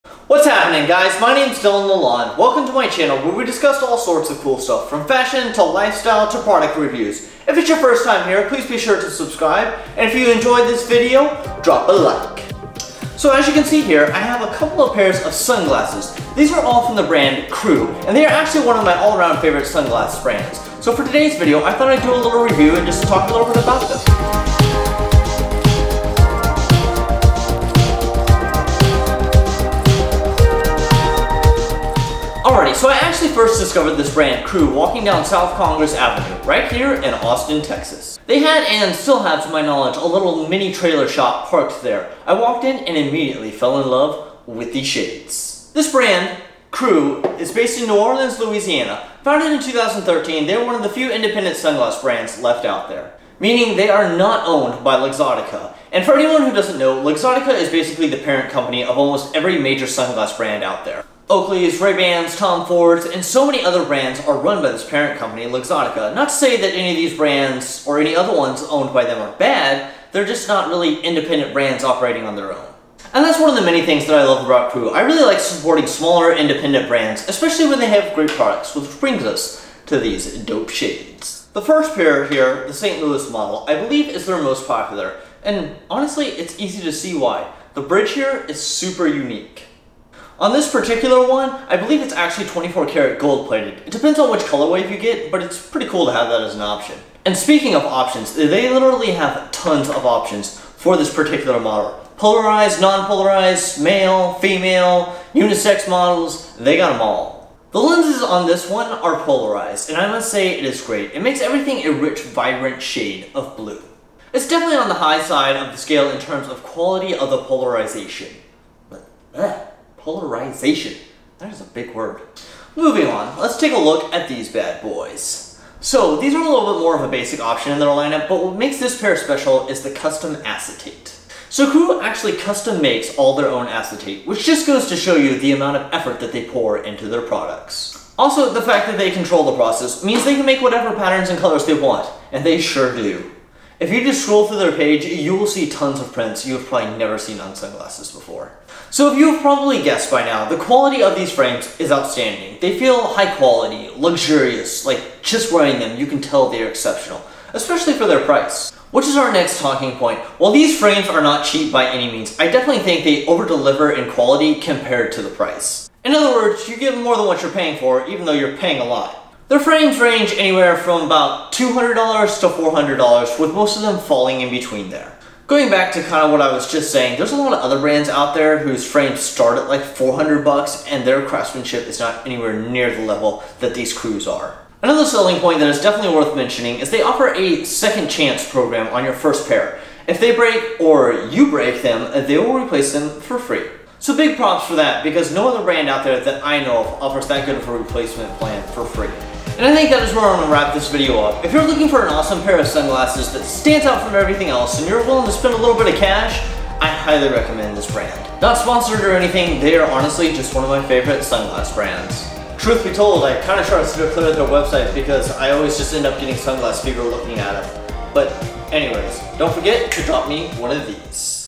Krewe Sunglasses Review & Honest Opinion
How it going guys?! in this video i share my thoughts, commits, and feelings on the brand Krewe and their sunglasses.

Thank you Everybody for Watching!!!